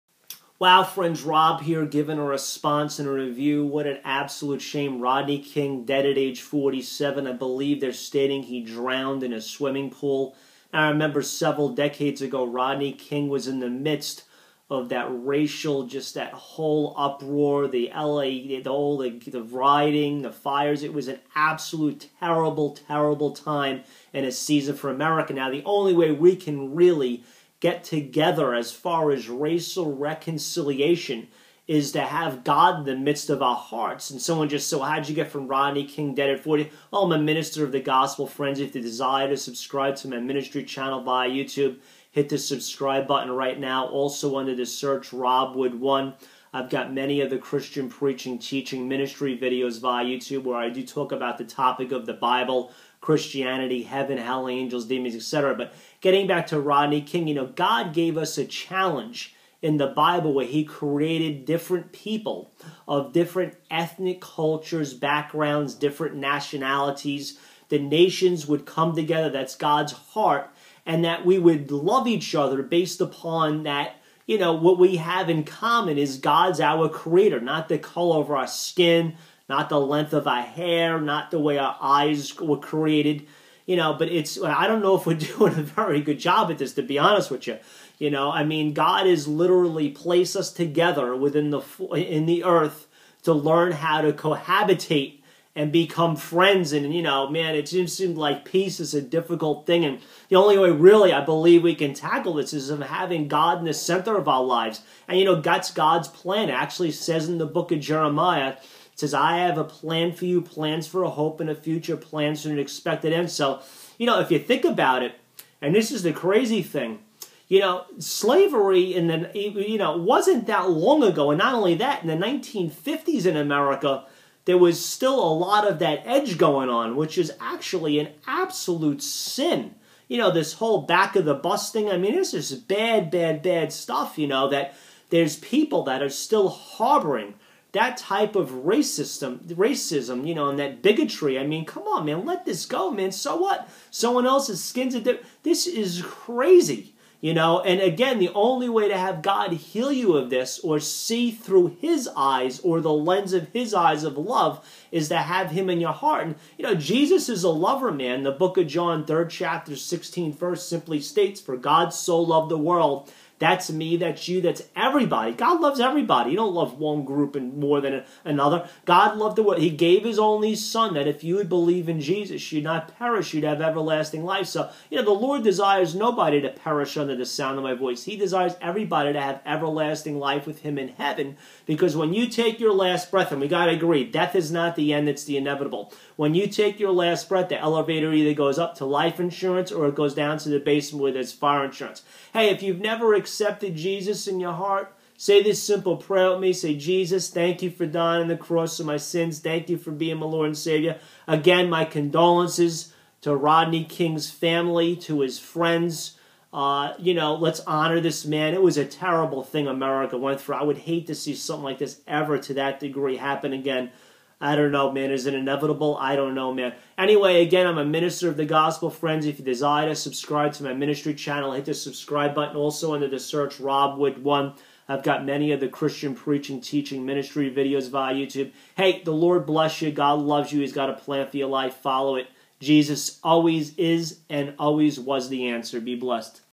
RODNEY KING FOUND DEAD IN POOL AT 47:Re
Robb shares about RODNEY KING AND ASKS HOW CAN WE SOLVE RACIAL  RECONCILIATION